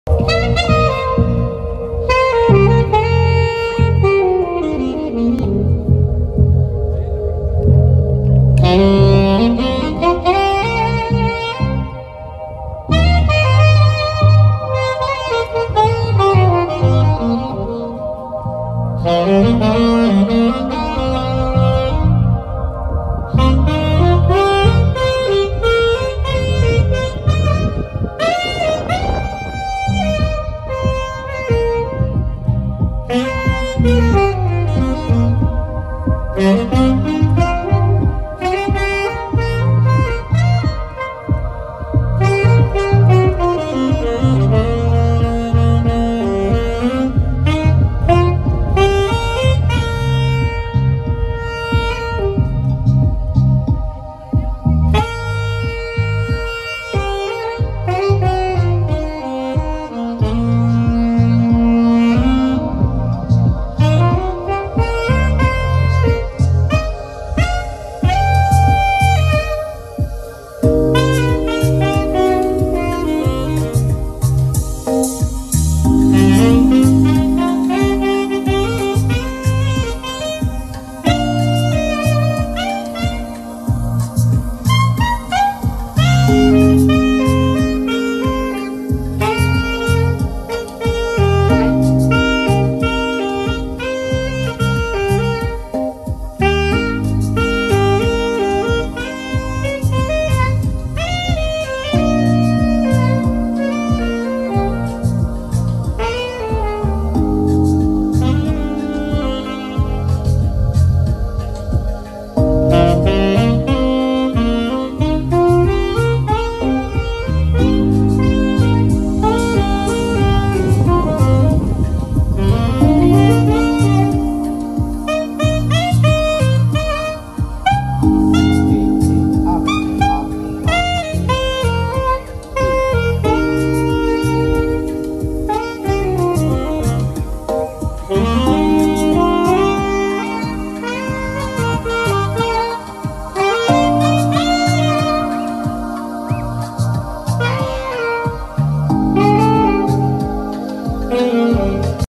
Sax Fusion - Agencia de Musicos CDMX
Saxofonista improvisa sobre base musical tipo lounge.